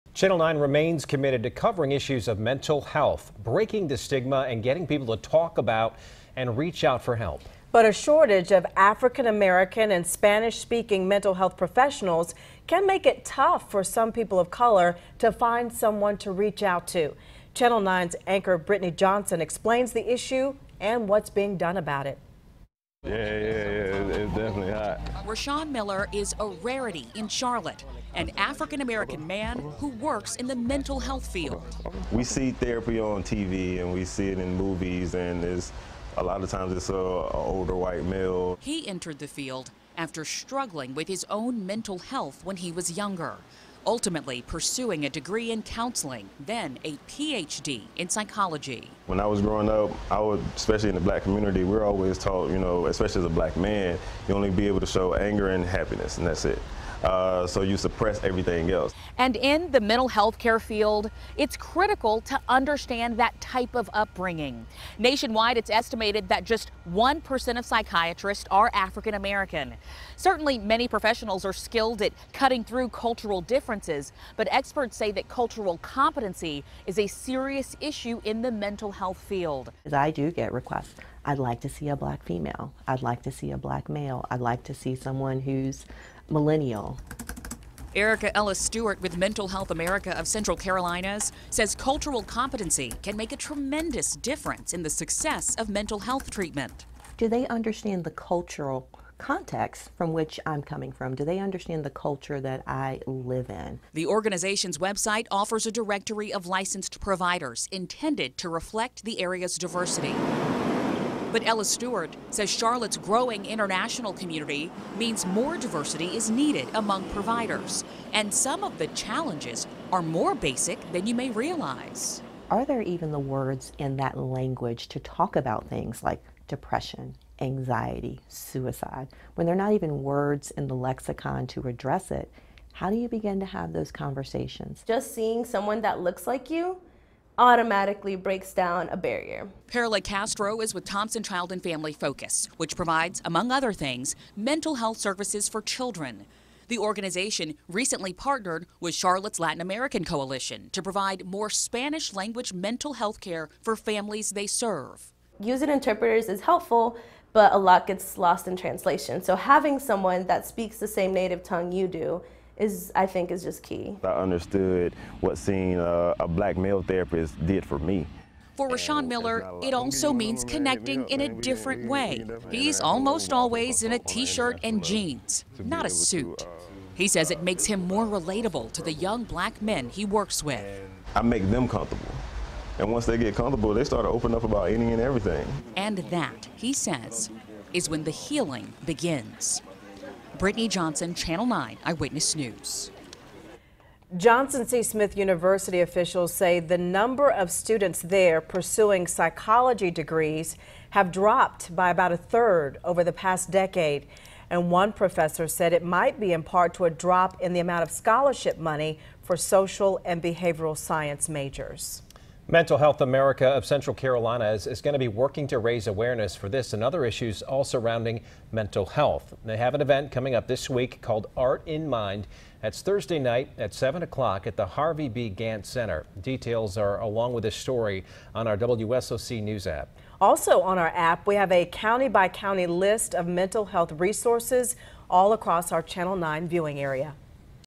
1% of psychiatrists are African-American and it's creating a gap in mental health care
1% of psychiatrists are African-American and it's creating a gap in mental health care